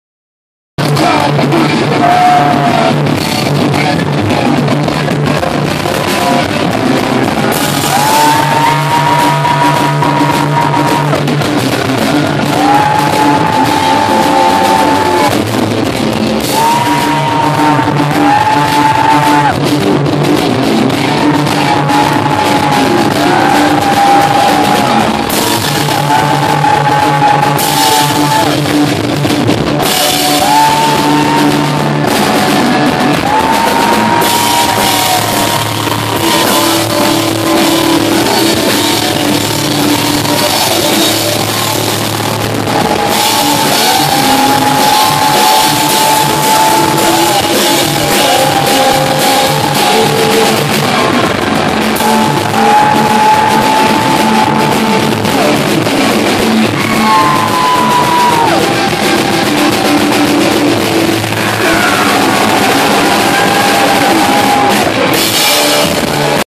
Deaner (Fubar) Shotgunning a beer in Ottawa
Deaner from Nightseeker (FUBAR) Shotgunning a Pistonead on stage at Mavericks in Ottawa April 26 2014